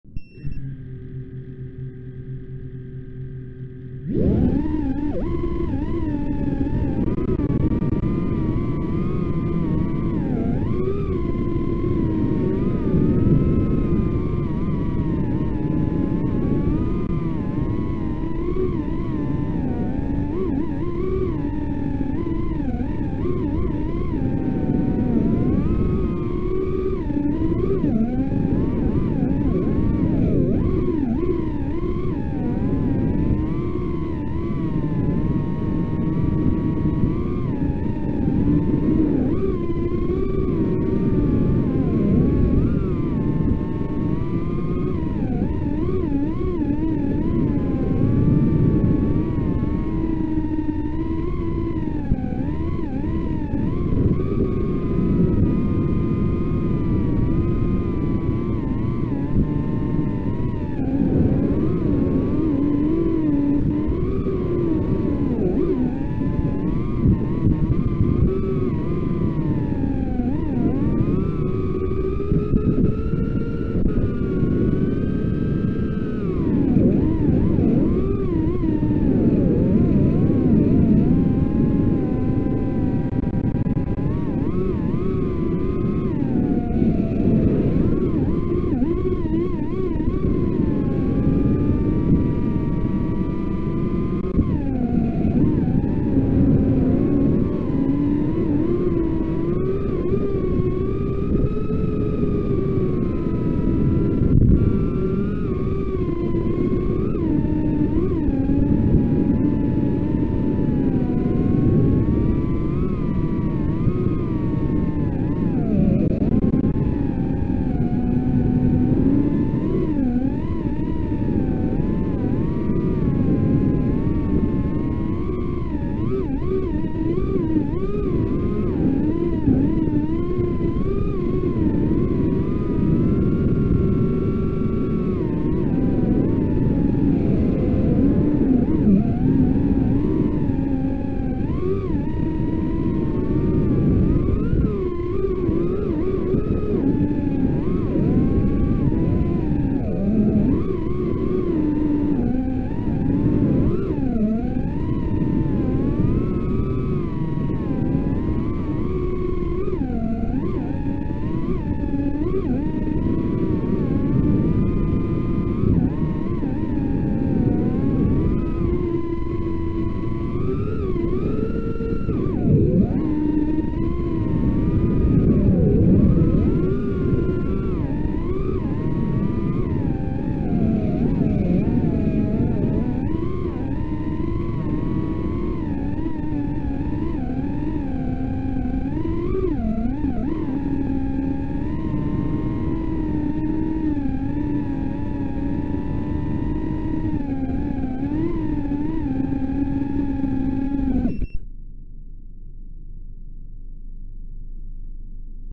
ELRS first flight, as far as I can slightly lose the motors sound
Casual first flight. Does it perform better than Ghost? maybe if we consider the receiver is equipped only with a small ceramic antenna.

betafpv elrs Micro TX with dynamic power (25 - 100mW) at 500Hz
BetaFPV RX Lite 2.4 GHz with ceramic tower antenna

Props: 3-inch
Frame: hglrc Sector 132
Motors: HGLRC Forward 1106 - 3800KV
FC + ESC: HGLRC F411 AIO, BLHeli_S flashed with the latest BlueJay at 48 KHz PWM frequency
Camera: caddx kangaroo
VTX: Zeus Nano at 25mW